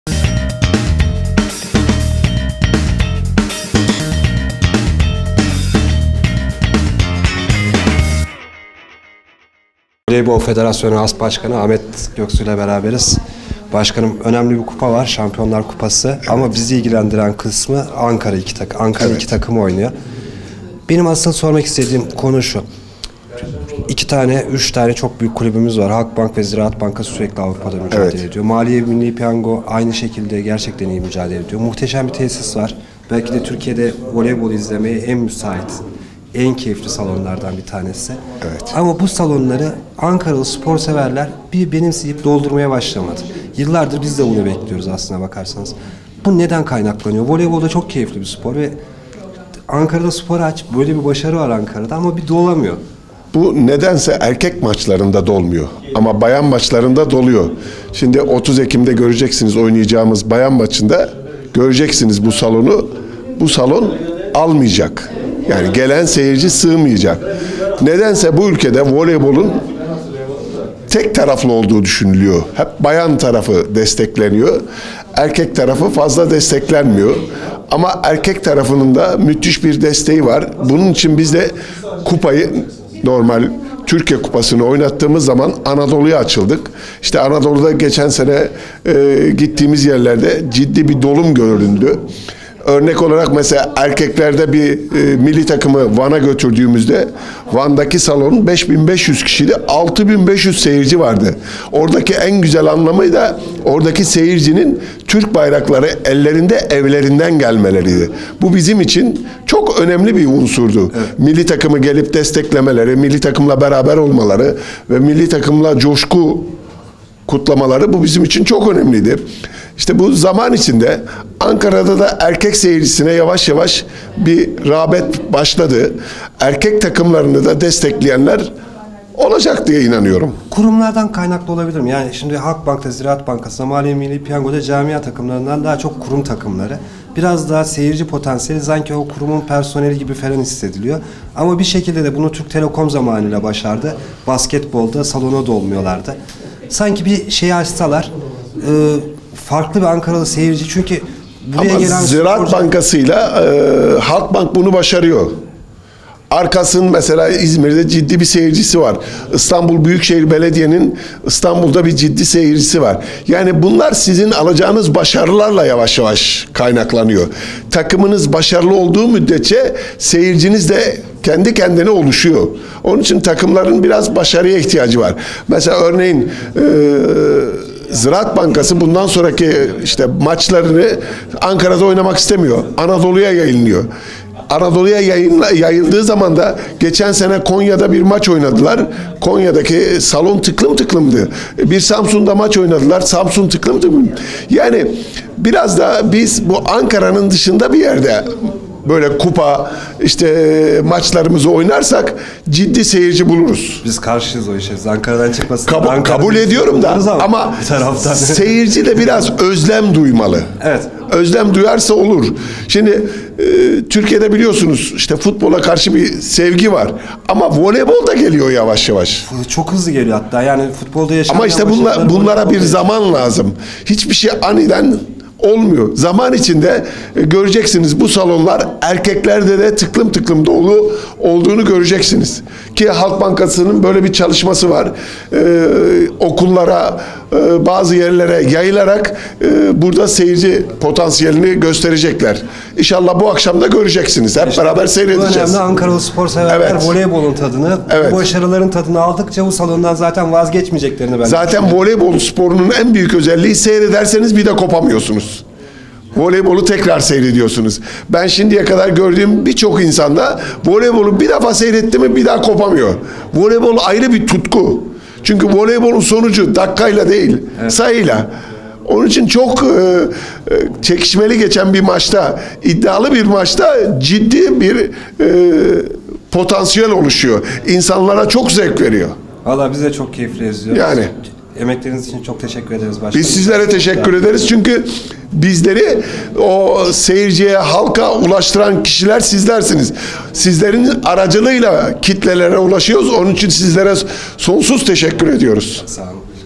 Voleybol Federasyonu Asbaşkanı Ahmet Göksu ile Ankara Voleybolunu konuştuk...
Ankara voleybolda şampiyonluklara doymuyor ama seyirciyi salona da çekmeyi beceremiyor. Bu sorunu Türkiye Voleybol Federasyonu Asbaşkanı Ahmet Göksu'ya sorduk...